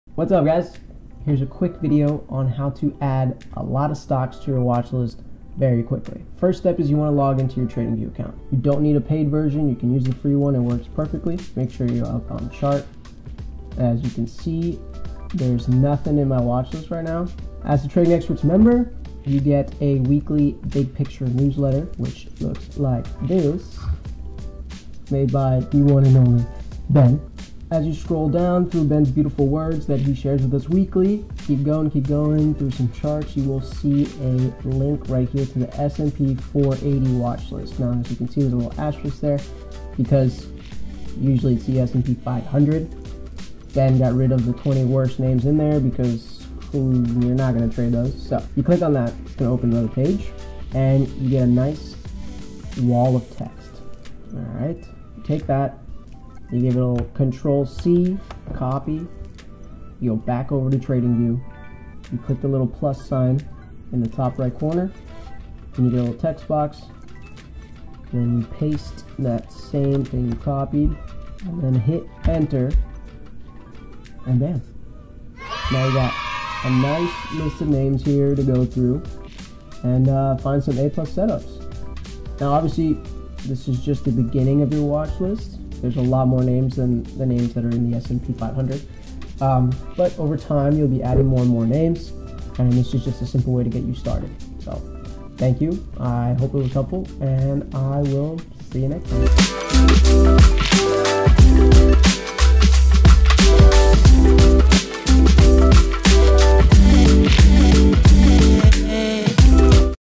Getting Started Lesson 7 Part 2 - How To Create Your S&P 500 Watchlist Into Trading View in Seconds!
Copy this text and add it to your watchlist

Copy and paste below into your TradingView Playlist for your S&P480 Watchlist

To receive our WEEKLY Market Outlook emails!